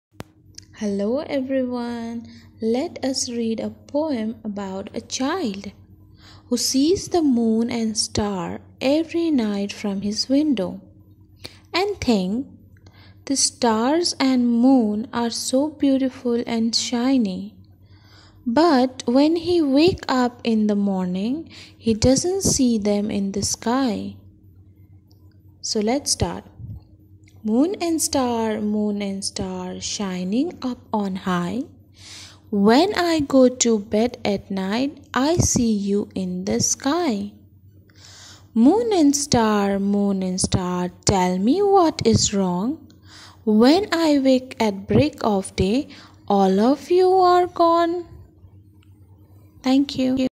Moon and star(poem)
Class 1st poem (moon and star). A child thinking why the moon and star are vanished during day.